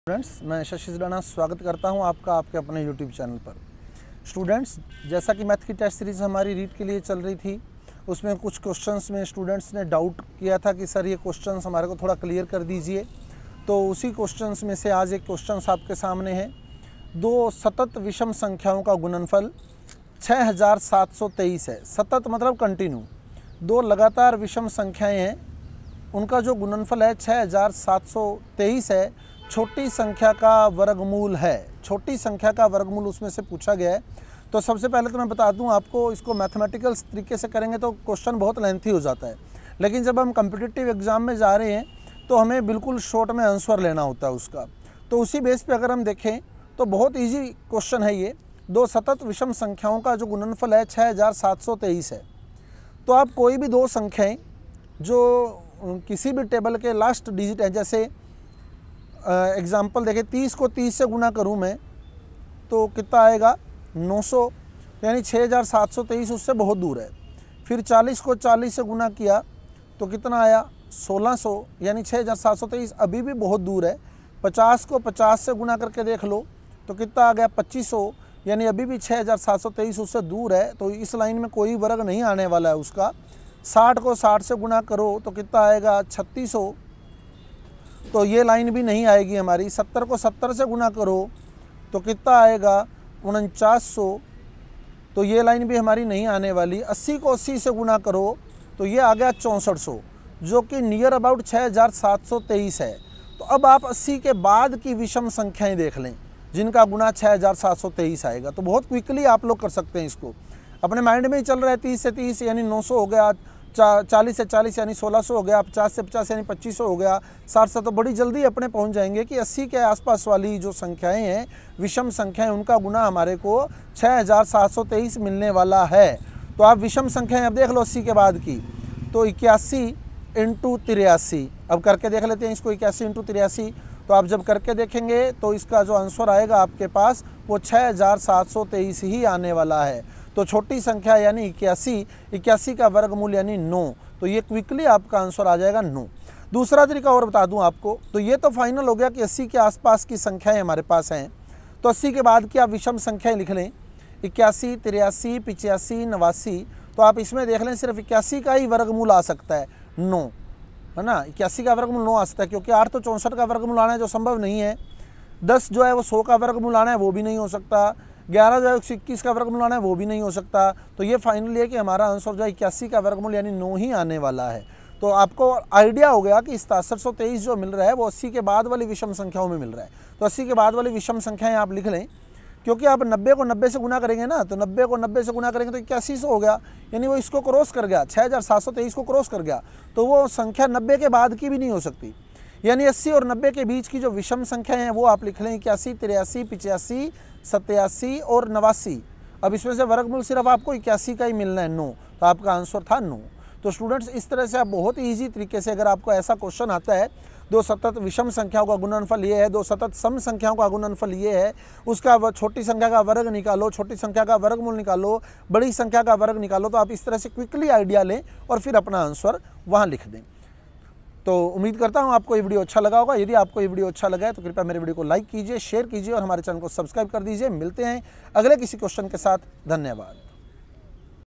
Maths trick||दो सतत विषम संख्याओं का गुणनफल 6723 है तो छोटी संख्या का वर्गमूल क्या होगा?
Learn with trick